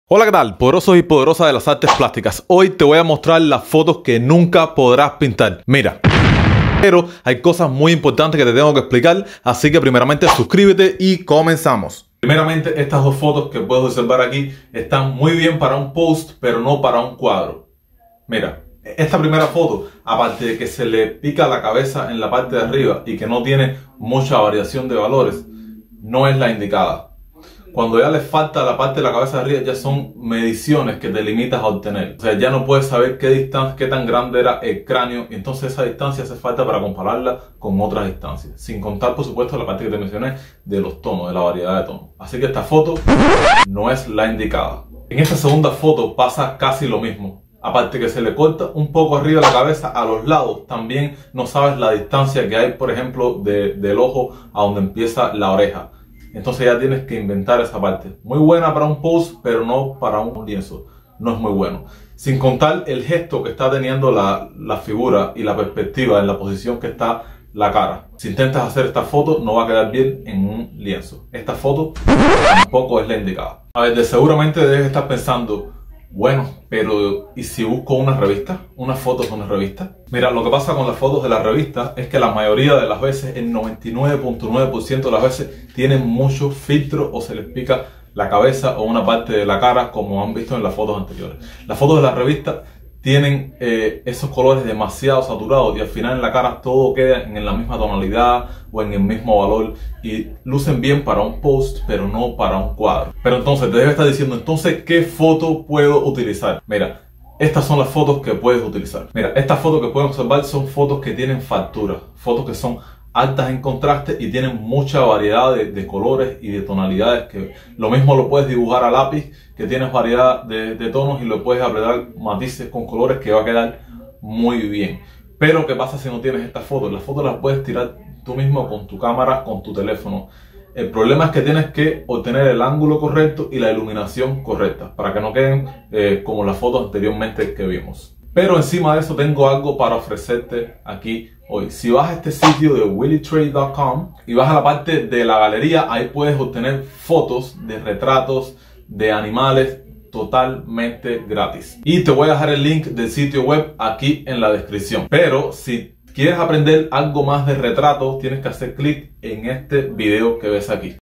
❌FOTOS que NO Puedes PINTAR. (El 99% Fracasa)😲​​... 2022
❌FOTOS que NO Puedes PINTAR. (El 99% Fracasa)😲​​... 2022.  Antes de pintar o dibujar es muy Importante saber que foto escoger en este video Descubriras la importancia que tiene eso, Aquí abajo te dejo el link del banco de fotos Gratuitas de nuestra Comunidad